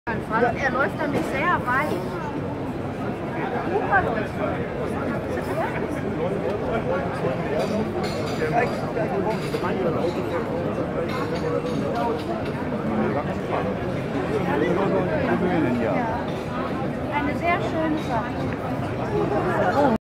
Roll-Go support walker walking aid # walk again # exco # invention
Dutch invented walking aid to avoid joint problems in arms or legs. No humping, ...drive your crutch!